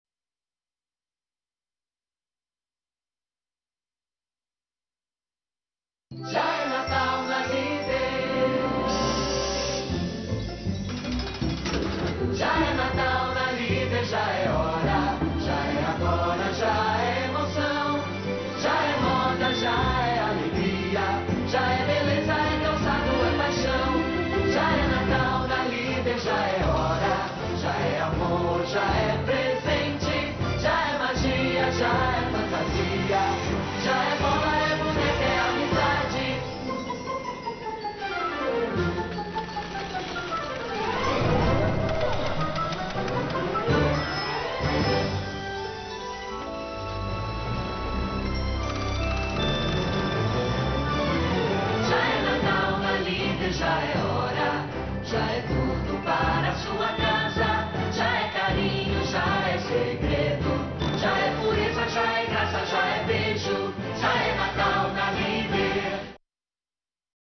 natal_vermelhos
fizemos os aderecos em estilo " steam punk" para os figurinos do comercial da Leader produzido pela Zeppelin